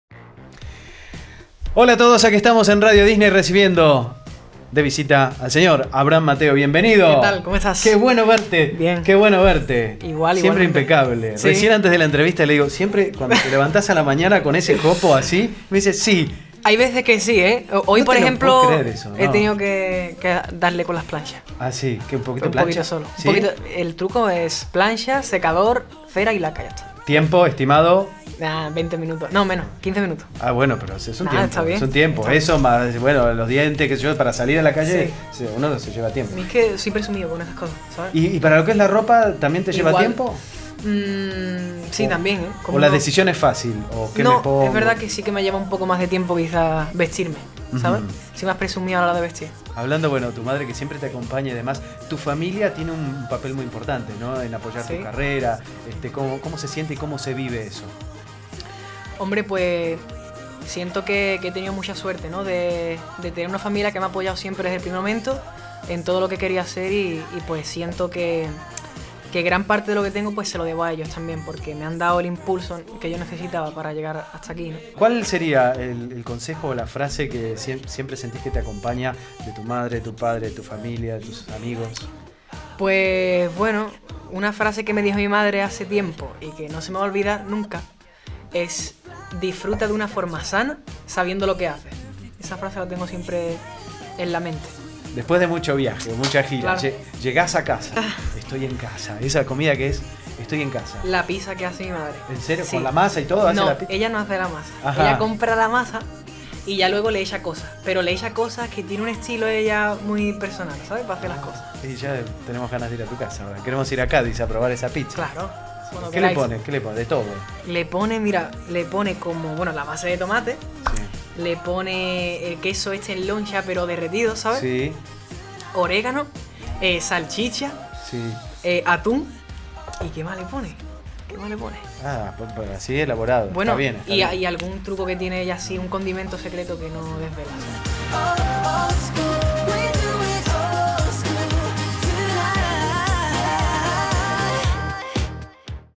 Abraham Mateo - Entrevista I
¿Cuál es la comida que lo hace sentir más en casa? No te pierdas la entrevista para saber qué respondió Abraham Mateo en entrevista con Radio Disney.